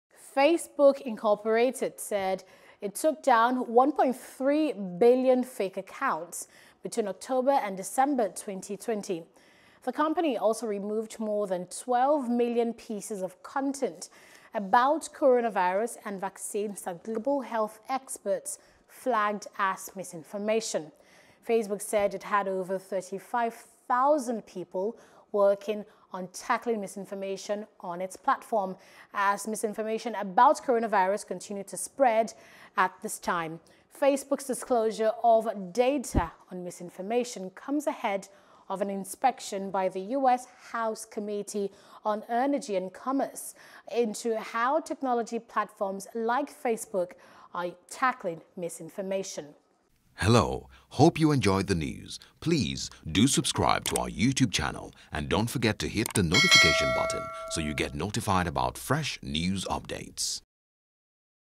Tackling Misinformation: Facebook Took Down 1.3 Billion Fake Accounts In Oct-Dec | NEWS
Facebook Incorporated said it took down 1.3 billion fake accounts between October and December 2020.

The company also removed more than 12 million pieces of content about COVID-19 and vaccines that global health experts flagged as misinformation.